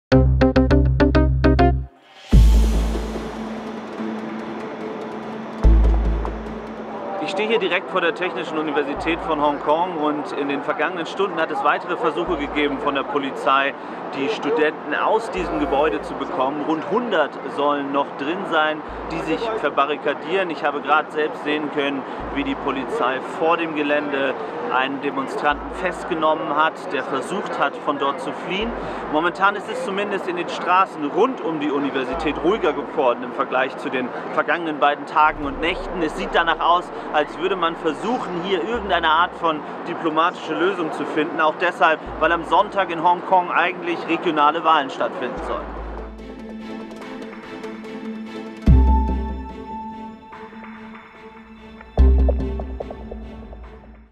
Tagelange Kämpfe mit der Polizei: Noch immer hundert Studenten in Uni Hongkong verbarrikadiert
Jetzt – nach Tagen der Kämpfe – scheint sich der Belagerungszustand an der Polytechnischen Universität in Hongkong langsam aufzulösen. Noch seien etwa 100 Demonstranten in der Uni, die nicht bereit wären aufzugeben – doch auch sie müssen zermürbt sein von der Gewalt. BILD-Chefreporter Paul Ronzheimer berichtet von der Lage vor Ort.